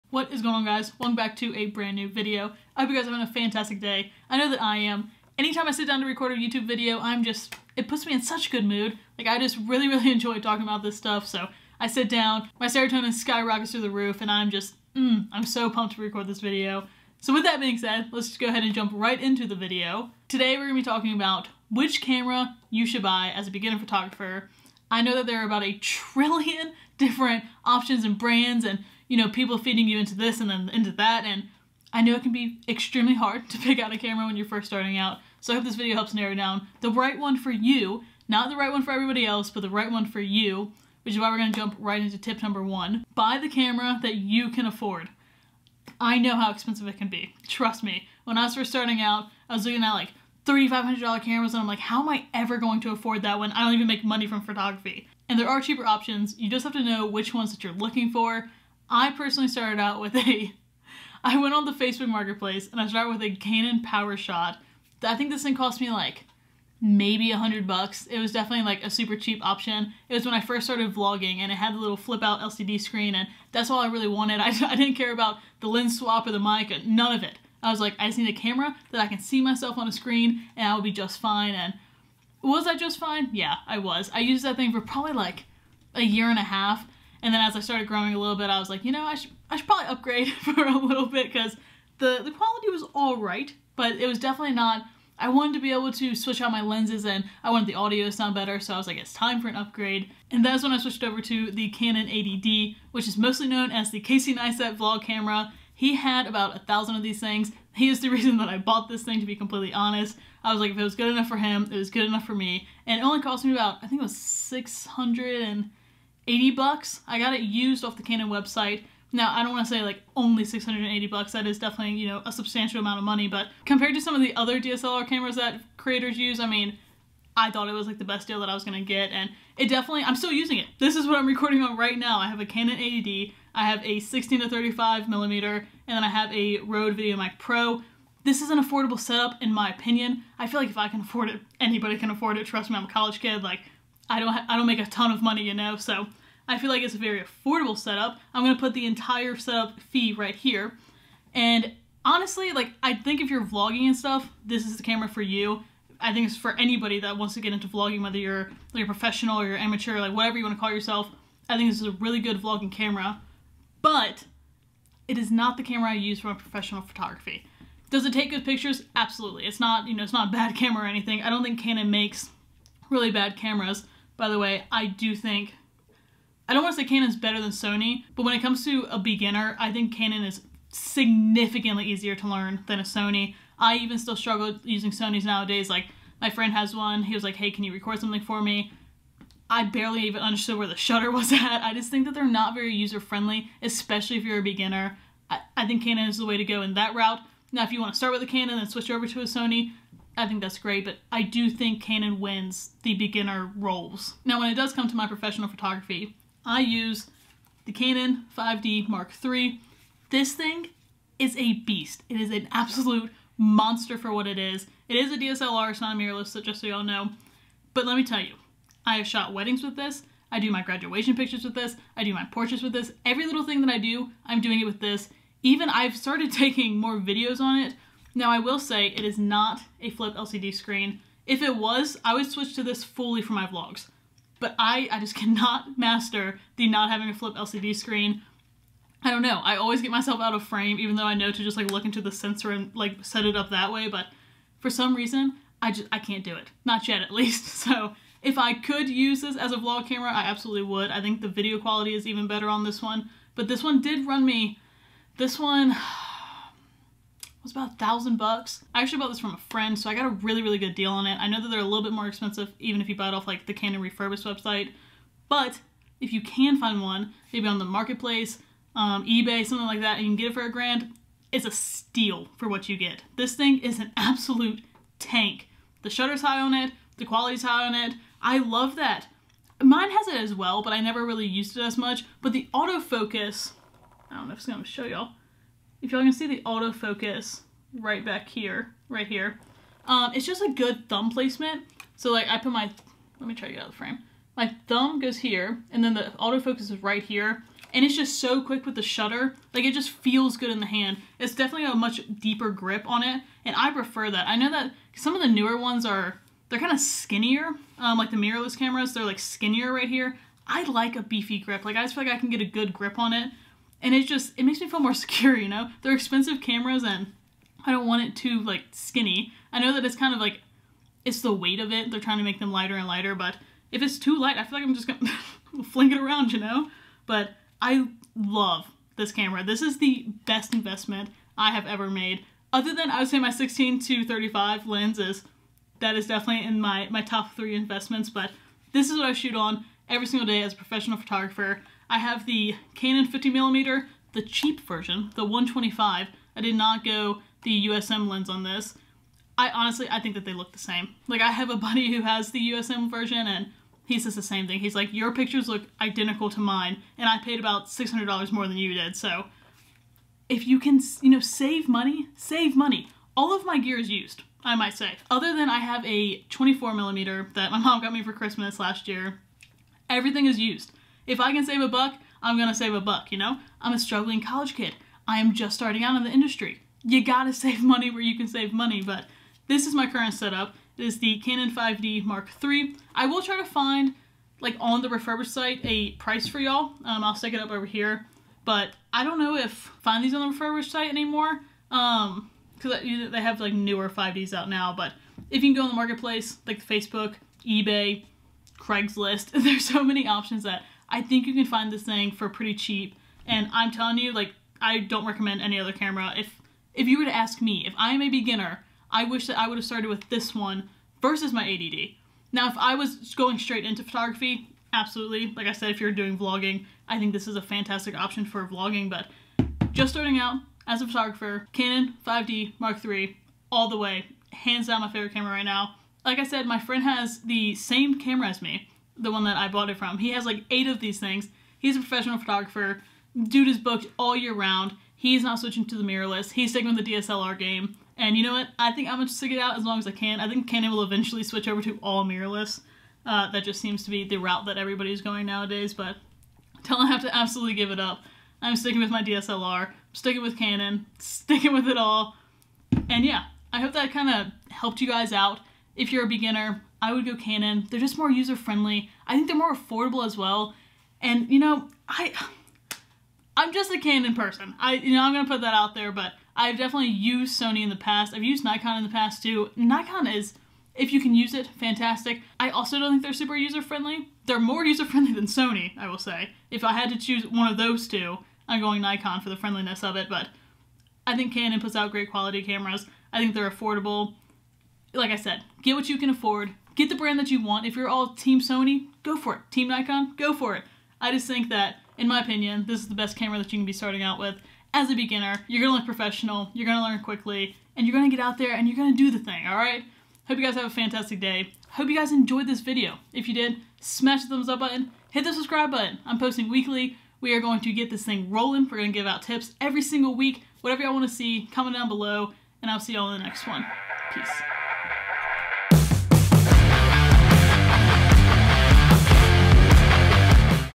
Best Beginner Camera in 2022 | DSLR cameras for beginners | Best camera for photography
What is going on guys? In today's video I am going to be telling you what the best beginner camera in 2022 is! (in my opinion) We will also be discussing the best DSLR cameras for beginners and overall the best camera for photography! This is going to be a super educational video and a super helpful one for beginner photographers! I hope you guys enjoy and take some information away from this video!

Camera I use: Canon 80D
Lens I use: Canon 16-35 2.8
Microphone I use: Rode video mic go
Tripod I use: Joby 5k